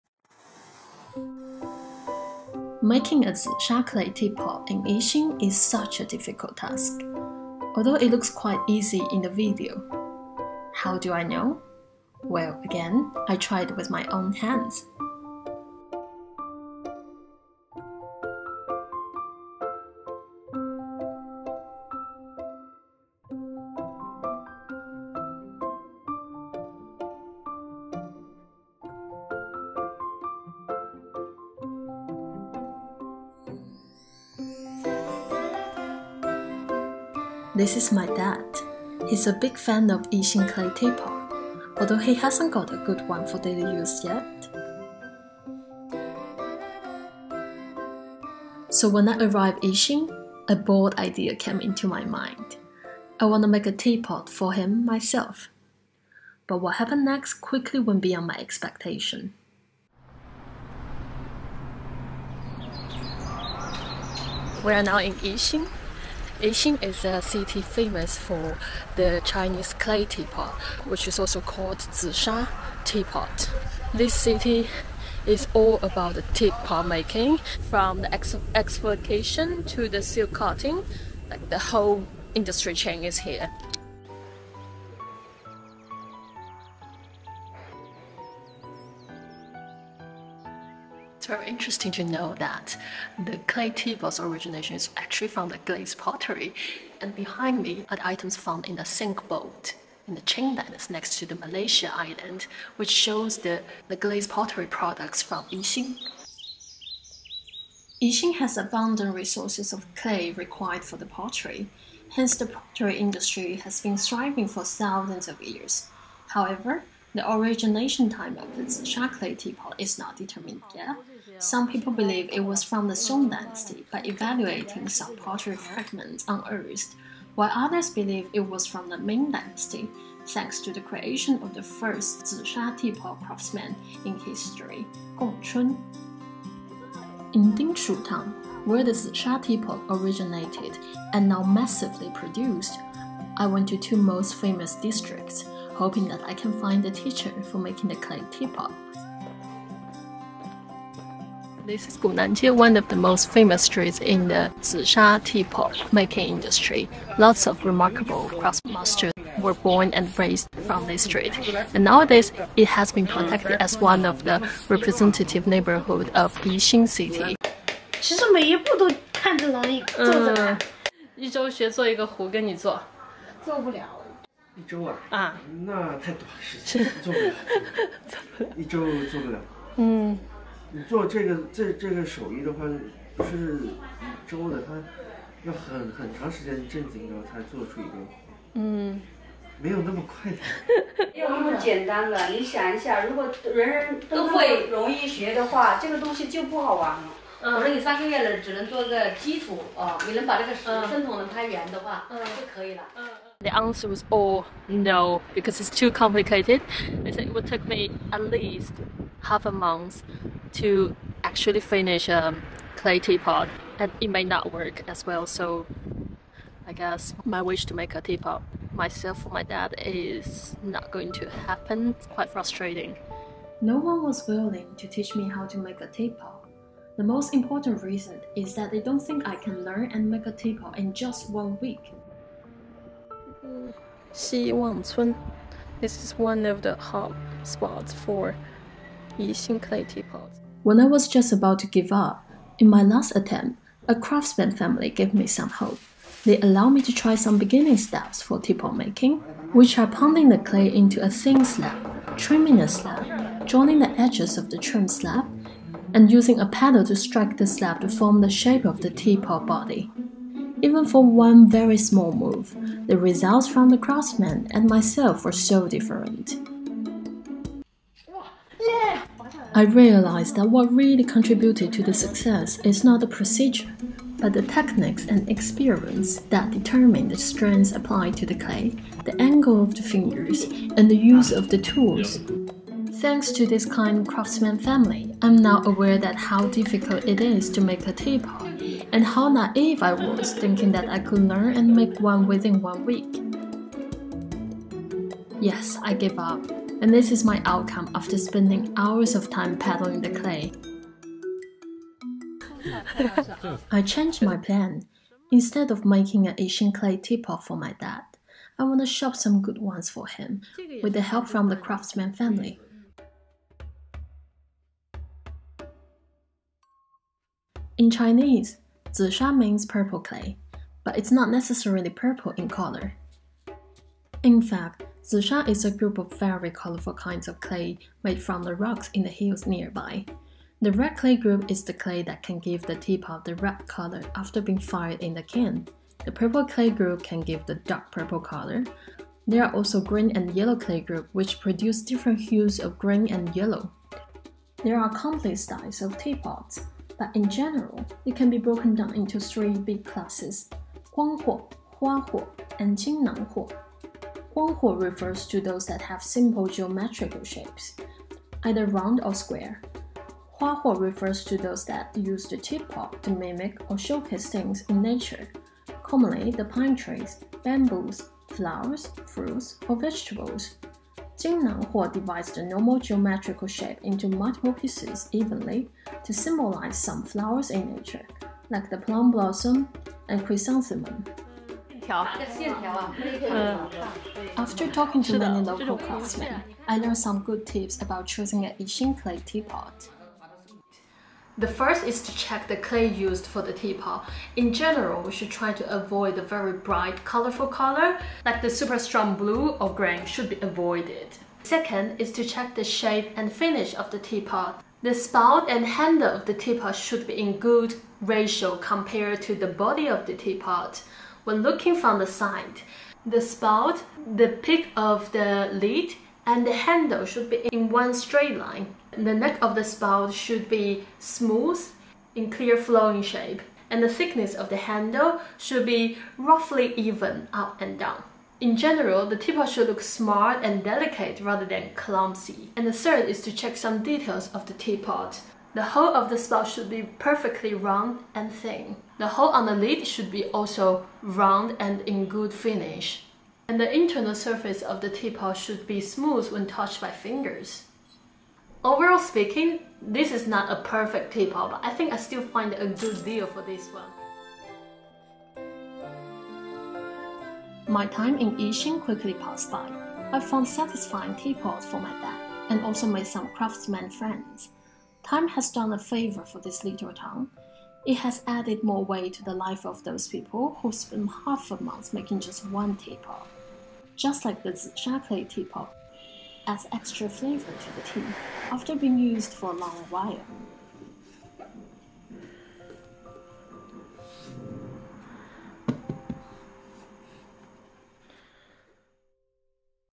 Challenge to make this teapot in 1 week, did it work?｜学习制作紫砂壶
This week I went to Yixing, another city in Jiangsu Province, China.

Yixing is a city famous for the Yixing clay teapot, also named Chinese zisha teapot. As my dad is a HUGE fan of tea, I wanted to make a tea for him from scratch. However, the story went wild...

In this video, you will join me looking for a teacher for making Yixing clay teapot, and learn how to choose a good teapot in the end.